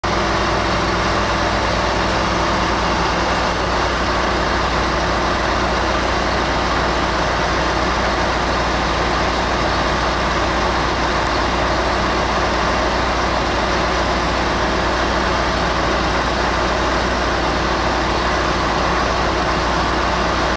2015.5.13 台鐵 平溪線 更換鋼梁 十分 新舊鐵橋暫存場作業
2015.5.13 台鐵 平溪線 更換鋼梁 十分 新舊鐵橋暫存場作業